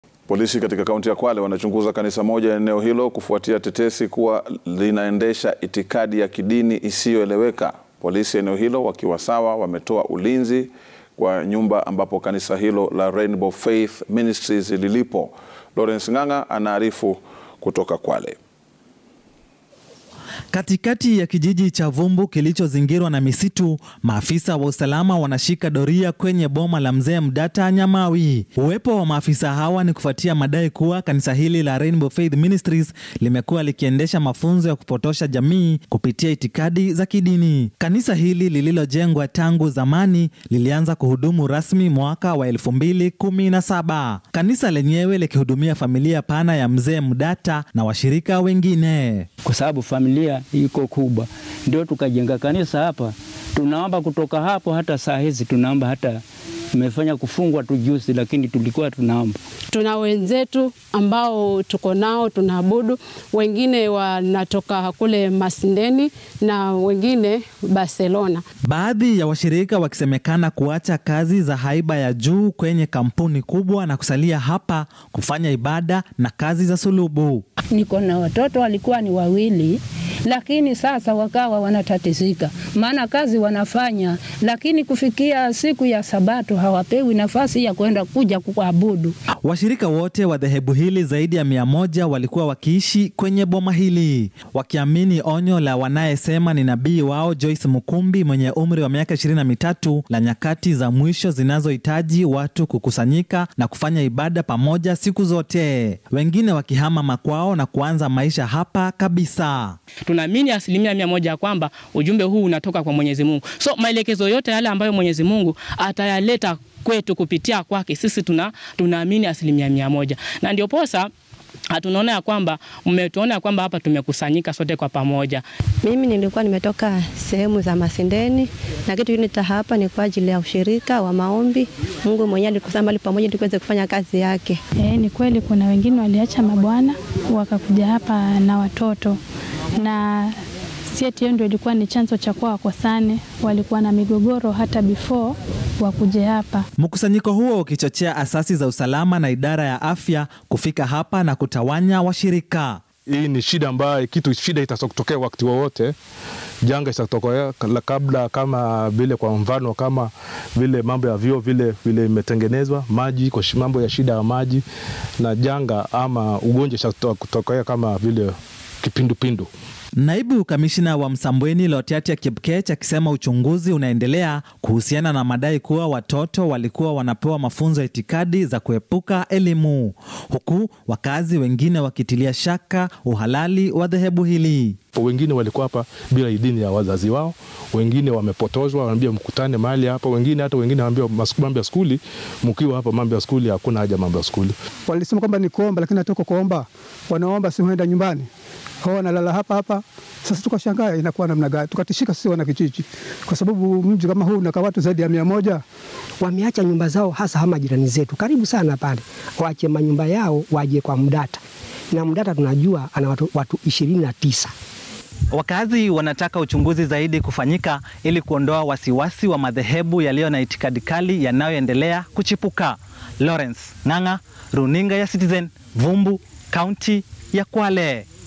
Polisi yafunga kanisa la 'Rainbow Faith Ministries' kwa kuendesha itikadi kali Kwale
Polisi kaunti ya Kwale wanachunguza kanisa moja eneo hilo, kufuatia tetesi kuwa zinaendesha itikadi ya kidini isiyoeleweka. Polisi eneo hilo wakiwa sasa wametoa ulinzi kwa boma ambapo kanisa hilo la Rainbow Faith Ministries lilipo. Lawrence Ng'ang'a anaarifu kuhusu mamia ya waumini waliokuwa wakiishi kwenye boma moja